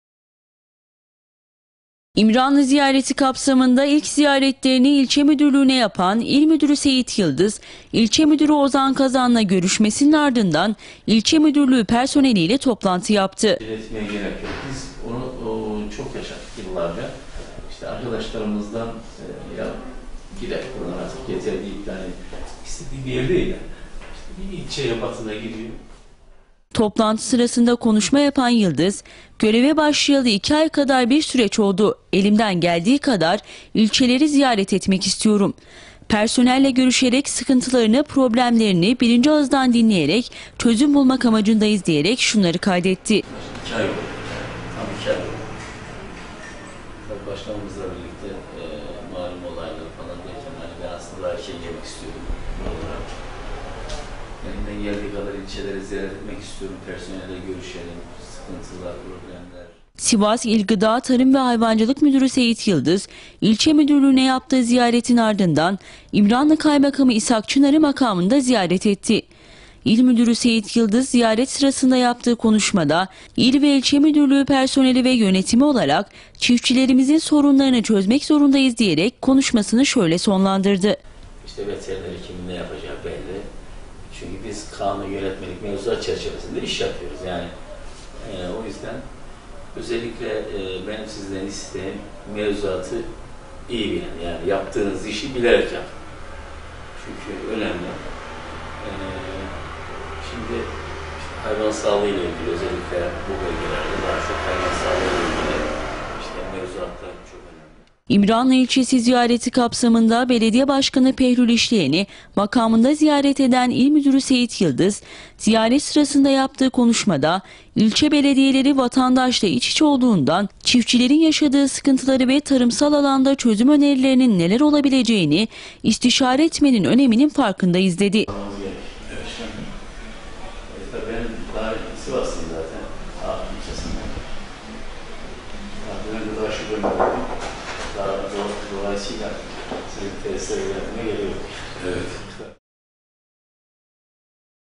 17 AĞUSTOS İL MÜDÜRÜ SEYİT YILDIZ İLÇE ZİYARETLERİNDE KAPSAMINDA İMRANLIDAYDI
SİVAS İL GIDA, TARIM VE HAYVANCILIK İL MÜDÜRÜ SEYİT YILDIZ İLÇE ZİYARETLERİ KAPSAMINDA DÖRDÜNCÜ ZİYARETİNİ İMRANLI İLÇESİNE YAPTI.